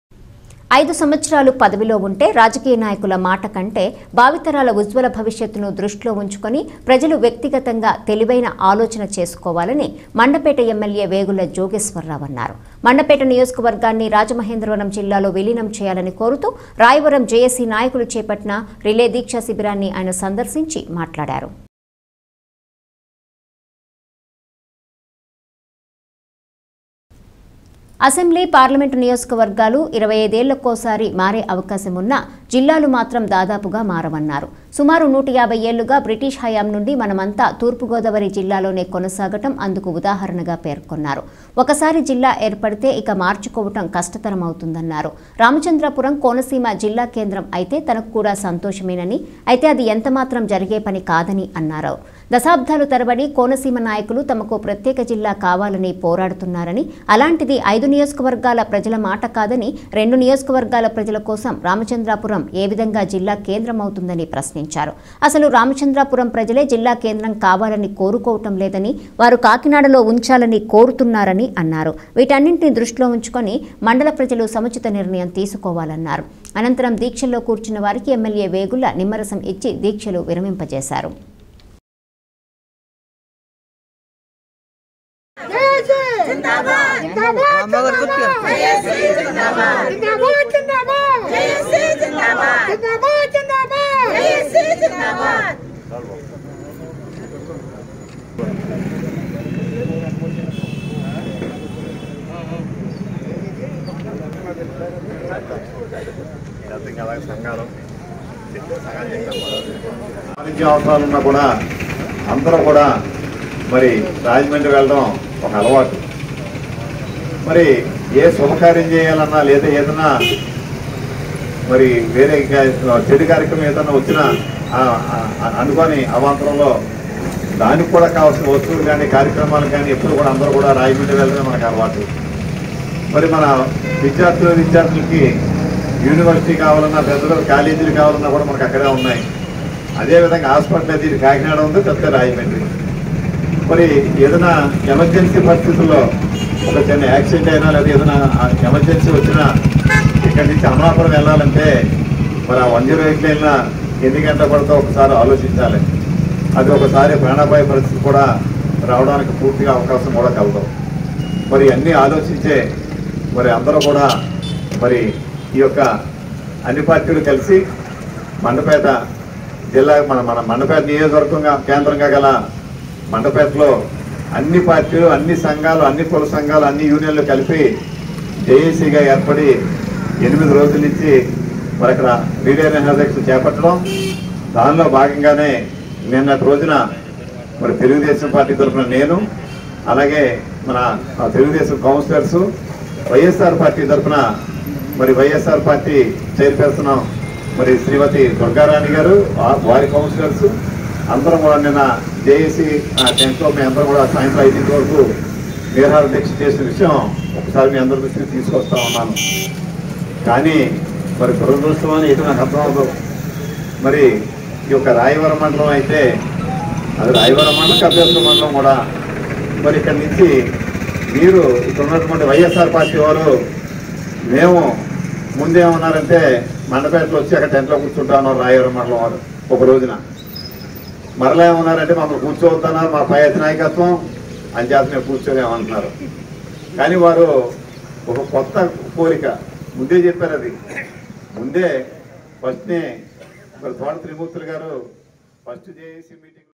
పార్లమెంట్ నియోజకవర్గాలు 25 ఏళ్లకోసారో మారే అవకాశం వున్నా జిల్లాలు మాత్రం దాదాపుగా మారవన్నారు.
అసెంబ్లీ , పార్లమెంట్ నియోజకవర్గాలు 25 ఏళ్లకోసారో మారే అవకాశం వున్నా జిల్లాలు మాత్రం దాదాపుగా మారవన్నారు. సుమారు 150 ఏళ్లుగా బ్రిటిష్ హయాం నుండి మనమంతా తూర్పు గోదావరి జిల్లాలోనే కొనసాగడం అందుకు ఉదాహరణగా పేర్కొన్నారు. ఒకసారి జిల్లా ఏర్పడితే ఇక మార్చుకోవడం కష్ట తరమవుతుందన్నారు. రామచంద్రాపురం కోనసీమ జిల్లా కేంద్రం అయితే తనకు కూడా సంతోషమేనని, అయితే అది ఎంతమాత్రం జరిగేపని కాదని అన్నారు. దశాబాలు తరబడి కోనసీమ నాయకులు తమకు ప్రత్యేక జిల్లా కావాలని పోరాడుతున్నారని, అలాంటిది అయిదు నియోజకవర్గాల ప్రజల మాట కాదని రెండు నియోజకవర్గాల ప్రజల కోసం రామచంద్రాపురం ఏ విధంగా జిల్లా కేంద్రమవుతుందని ప్రశ్నించారు. అసలు రామచంద్రాపురం ప్రజలే జిల్లా కేంద్రం కావాలని కోరుకోవడం లేదని, వారు కాకినాడ లో ఉంచాలని కోరుతున్నారని అన్నారు. వీటన్నింటినీ దృష్టిలో ఉంచుకుని మండల ప్రజలు సముచిత నిర్ణయం తీసుకోవాలన్నారు. .అనంతరం దీక్షల్లో కూర్చున్న వారికి ఎమ్మెల్యే వేగుళ్ల నిమ్మరసం ఇచ్చి దీక్షలు విరమింపజేశారు.